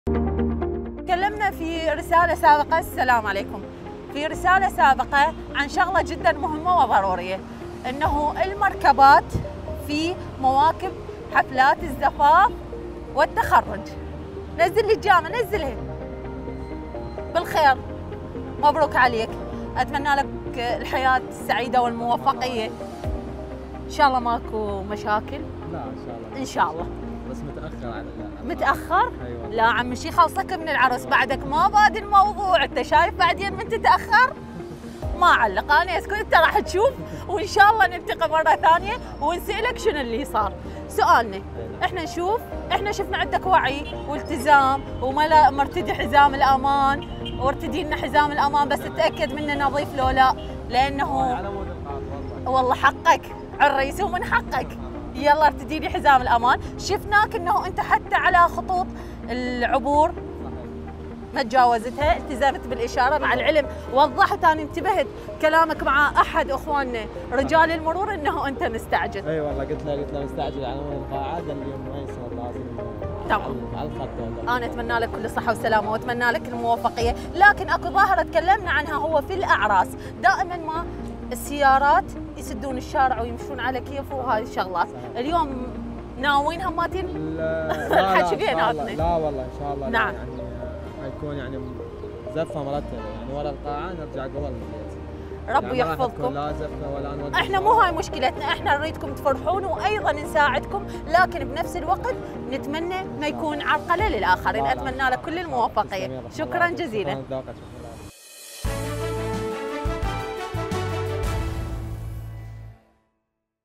شرطة المرور توقف عريساً عراقياً تأخر على الزفة لهذا السبب!
شرطة المرور توقف عريساً تأخر على الزفة لهذا السبب!
موقع قناة التغيير الفضائية  الالكتروني